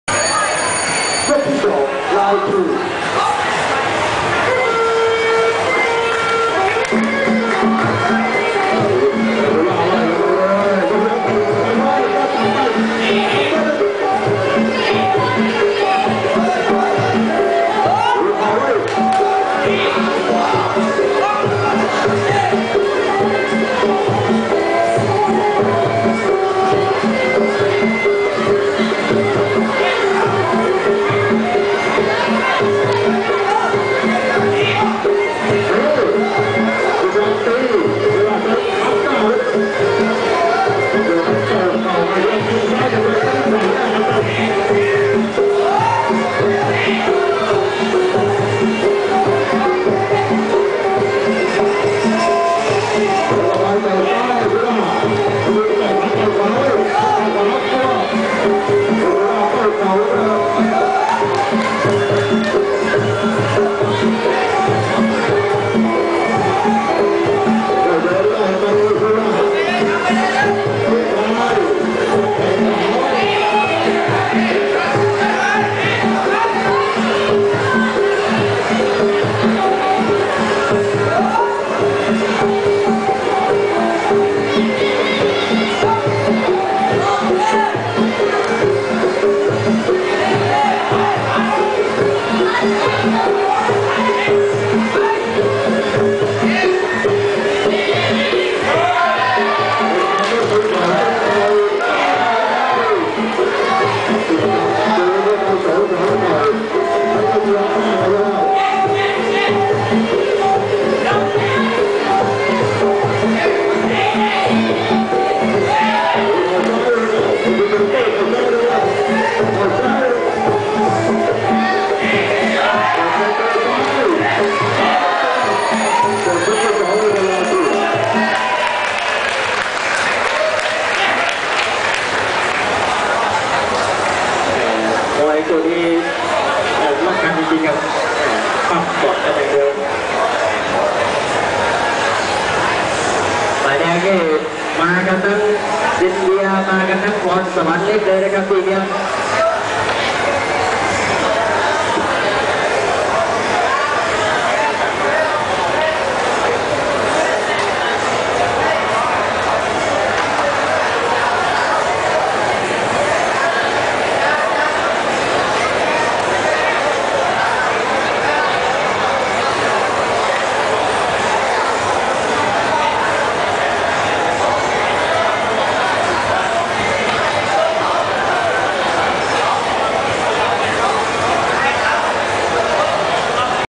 My First Thai Fight, Loi Kroh, Round 2 - Female Muay Thai, Lanna Camp
The second round of my first Thai fight at Loi Kroh Stadium.